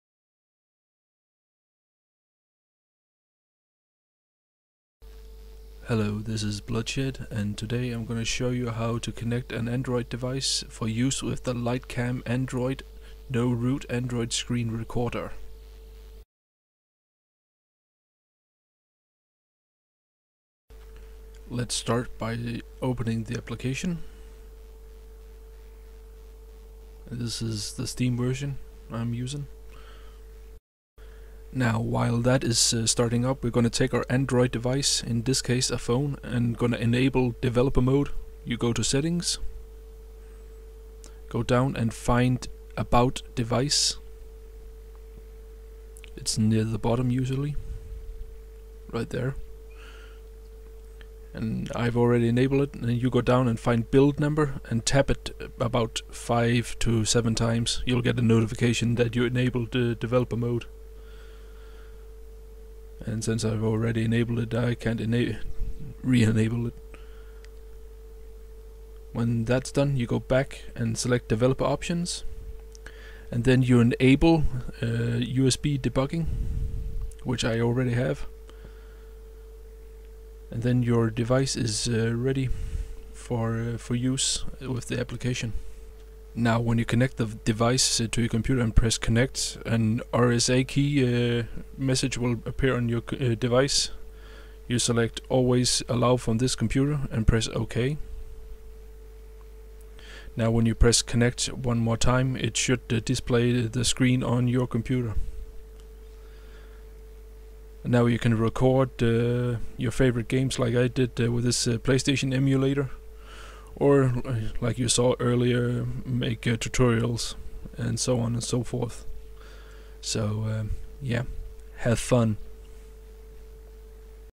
liteCam Android: No Root Android Screen Recorder - Connection Tutorial
This video show how to setup your Android device and connect it to liteCam Android to you can record videos of the device screen on your PC.